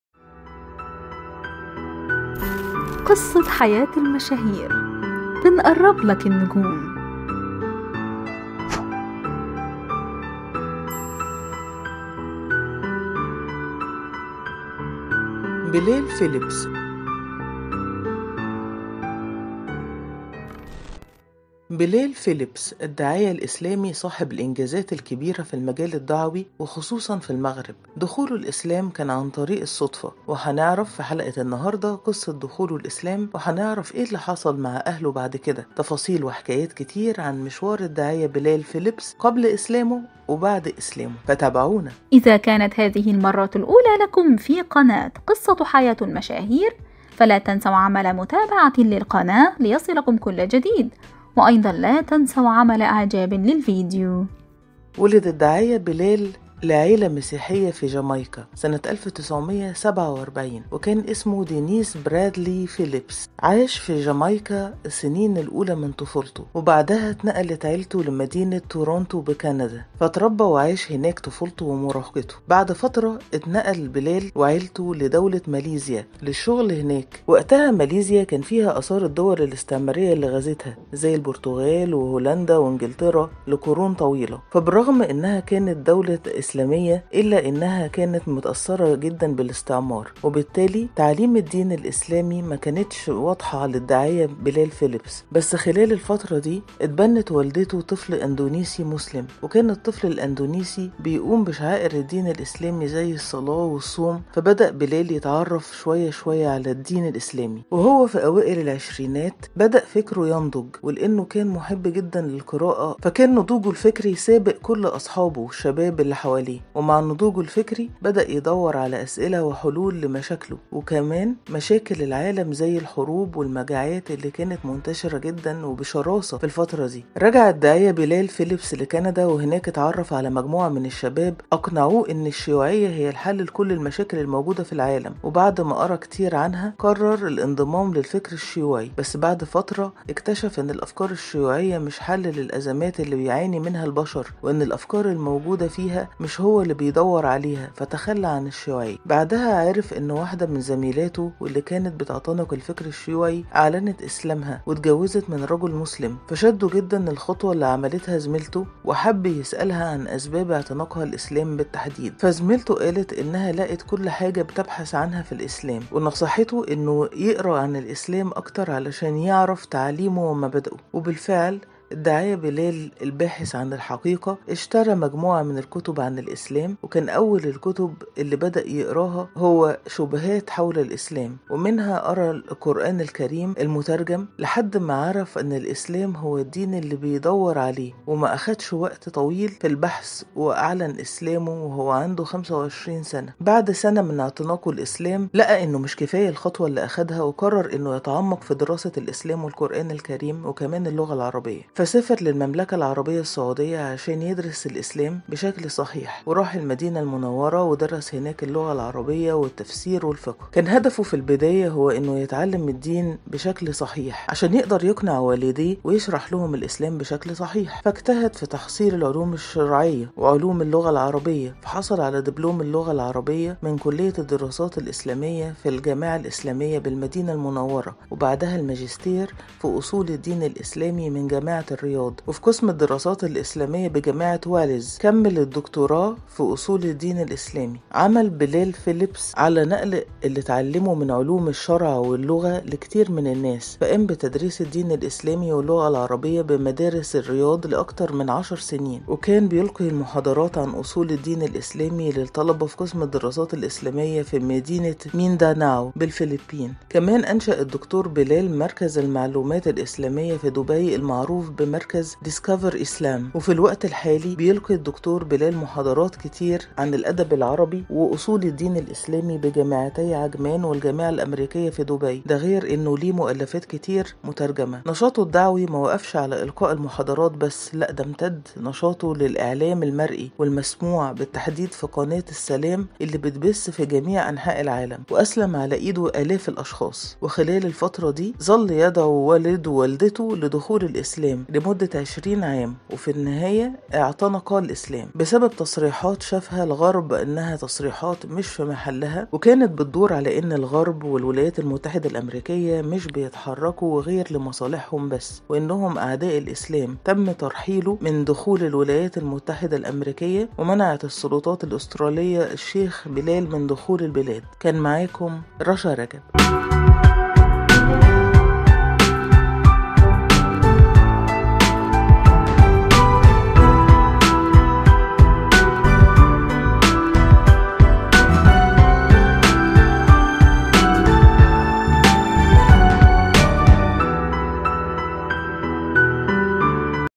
بلال فيلبس بحث عن حلول لمشاكله فوجد الاسلام وجه انتقادات للغرب فتم ترحيله من امريكا
بلال فيلبس بحث عن حلول لمشاكله فوجد الاسلام وجه انتقادات للغرب فتم ترحيله من امريكا - قصة حياة المشاهير بلال فيلبس الداعية الاسلامي صاحب الانجازات الكبيرة في المجال الدعوي وخصوصا في الغرب ، دخوله الاسلام كان عن طريق الصدفة و هنعرف في حلقة النهاردة قصة دخوله الاسلام وهنعرف ايه ال حصل مع اهله بعد كده ، تفاصيل وحكايات كتيرة عن مشوار الداعية بلال فيلبس قبل اسلامه وبعد اسلامه فتابعونا